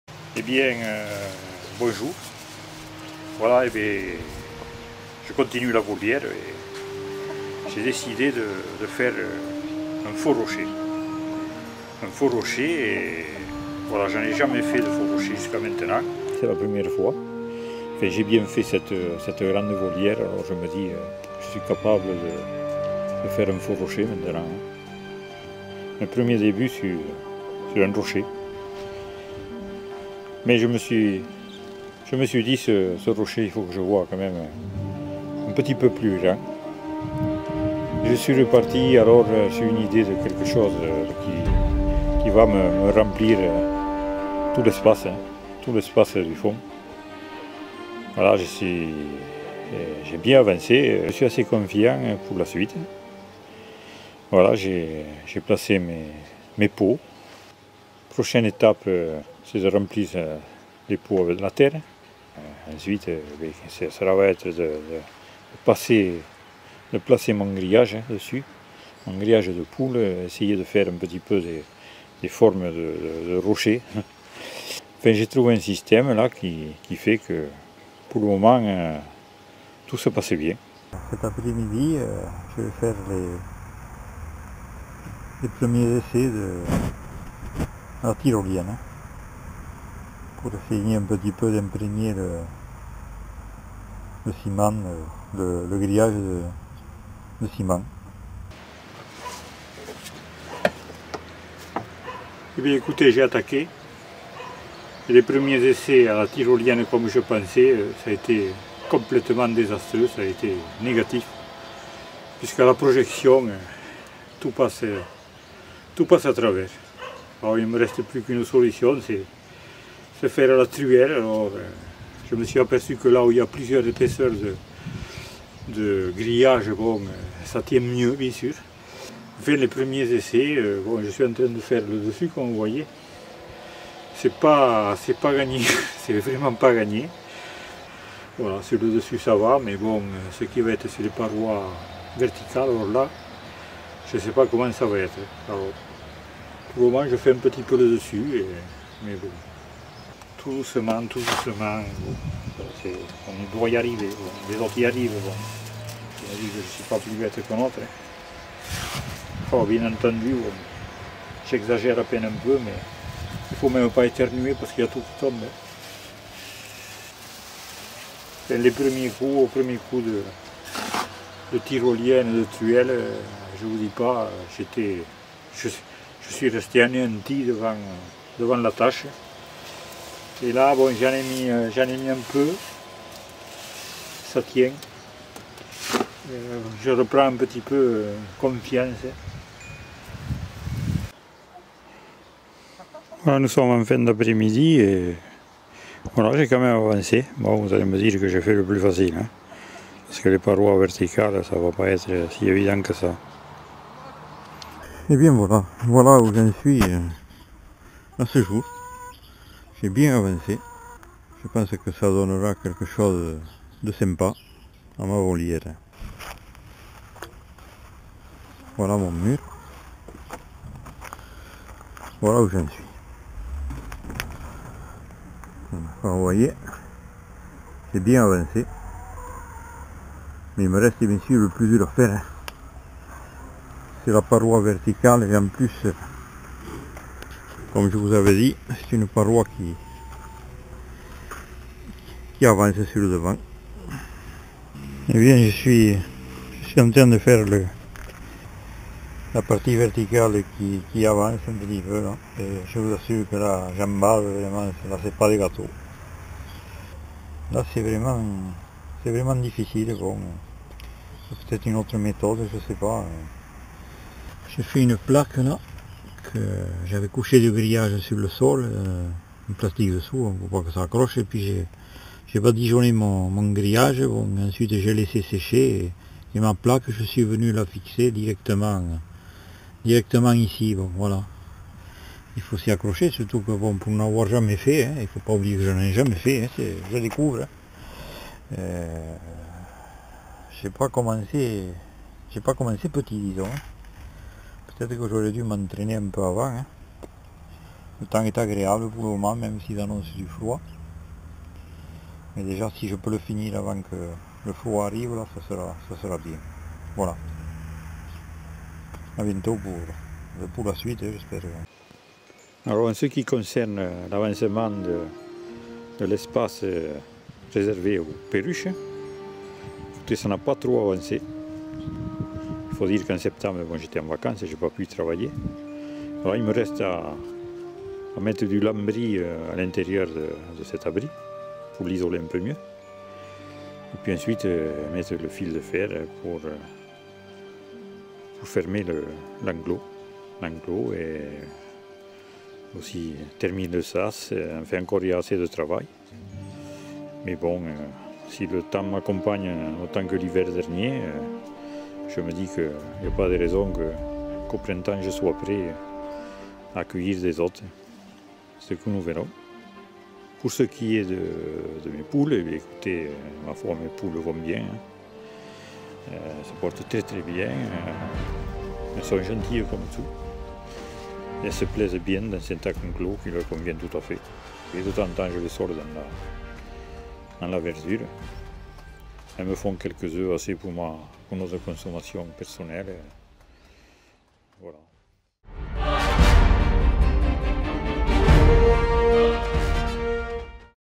Faux rocher dans ma volière (1)
J'ai décidé de construire un grand rocher dans ma volière, voici la 1ère partie - Réalisation volière
I decided to go, so I'll build a big rock in my aviary here is the first part I